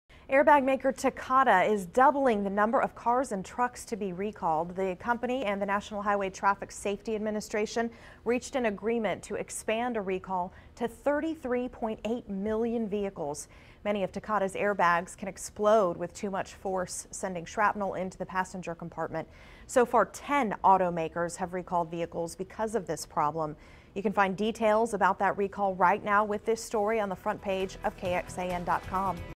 Air bag recall doubles to nearly 34 million
Under pressure from U.S. safety regulators, Takata Corp. has agreed to declare 33.8 million air bags defective, a move that will double the number of cars and trucks included in what is now the largest auto recall in U.S. history.